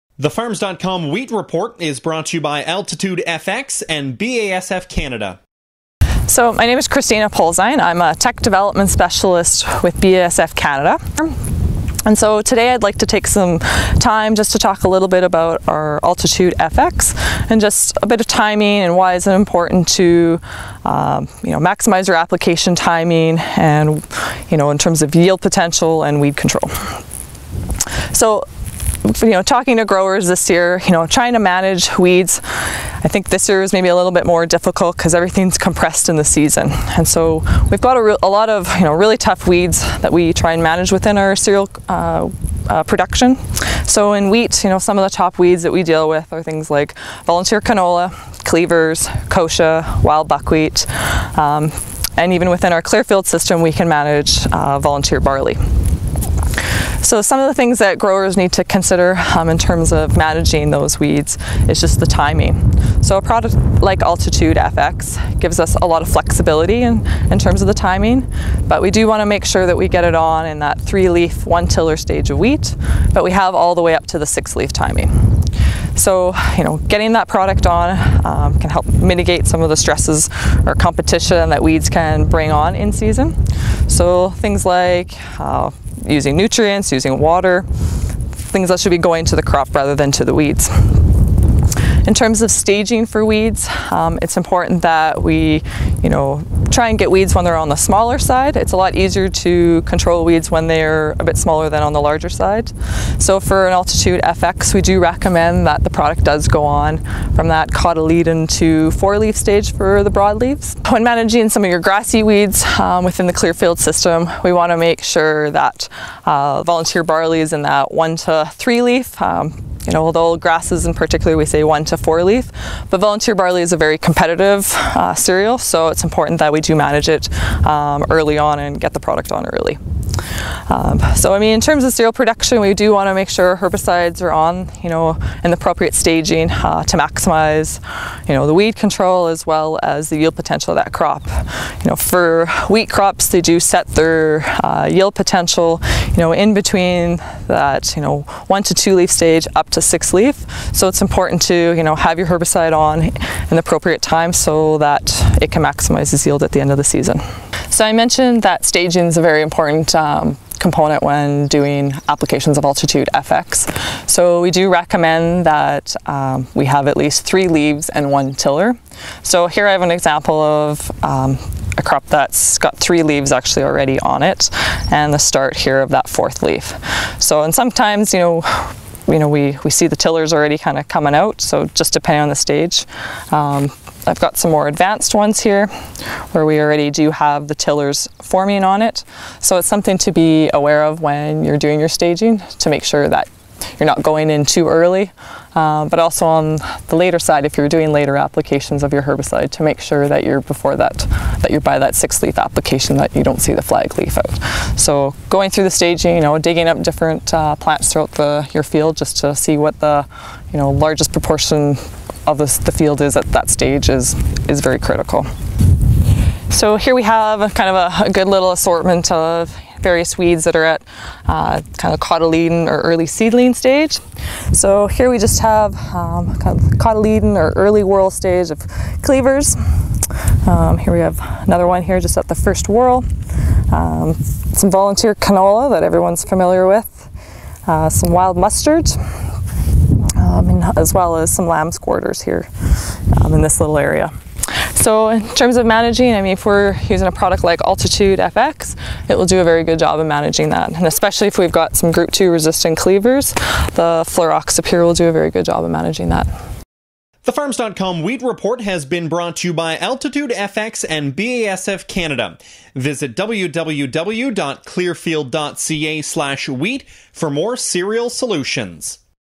Wheat Report: Controlling Tough Weeds with Altitude FX in Clearfield Production System for Wheat
Farms.com Wheat Report

Kristina shares some thoughts on how application timing and weed staging can improve weed control and maximize yields in the Clearfield Production System for Wheat.

To see additional Farms.com Wheat Report videos visit